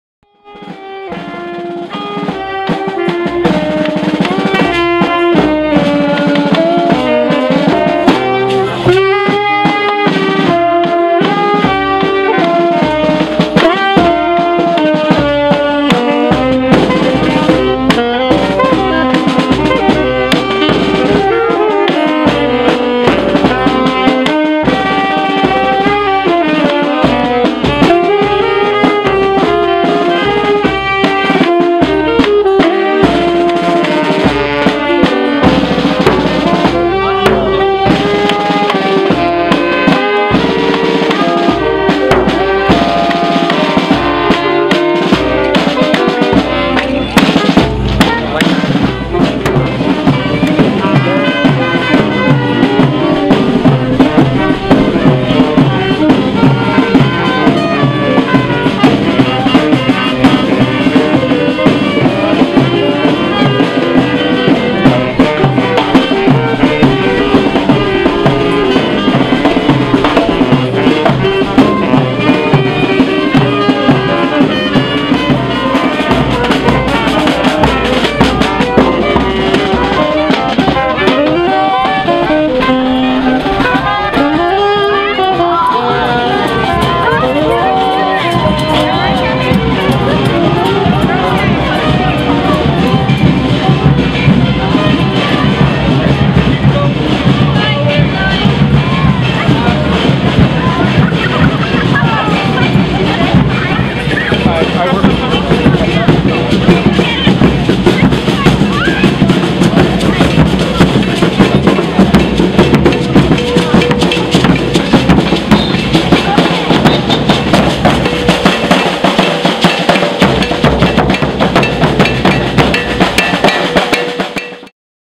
Night of Dread 2012 Parade Highlights
Night of Dread is Clay and Paper Theatre's annual parade and pageant of private and collective fears through the darkened streets of Toronto. Each year the neighbourhood procession of giant puppets, masks and costumes kicks of an evening that urges the local community to call on, mock and banish their fear.